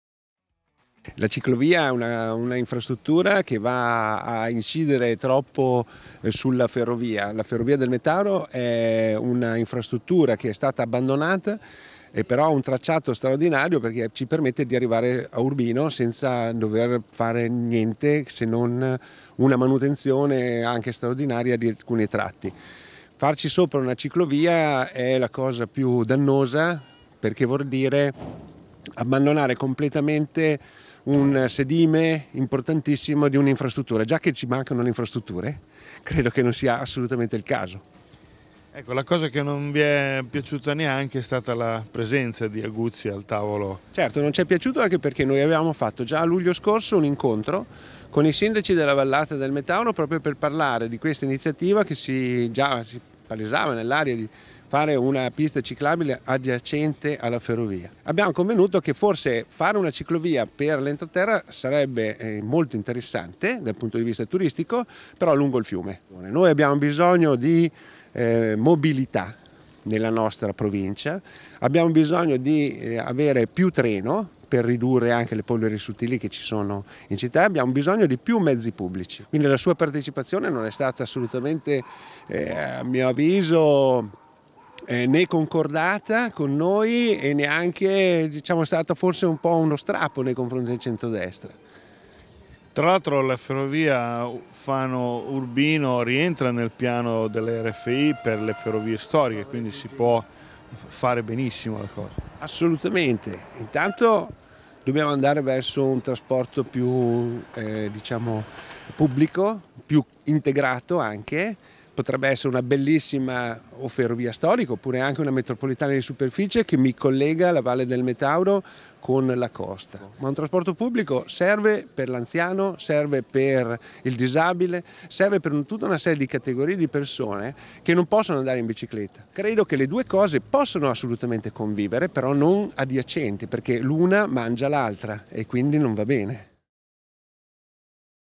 Lega Fano: "Realizzare ciclovia lungo il fiume"
La presentazione del progetto regionale della Ciclovia del Metauro prevista accanto al tracciato della ferrovia dismessa Fano Urbino, non è piaciuta affatto alla Lega.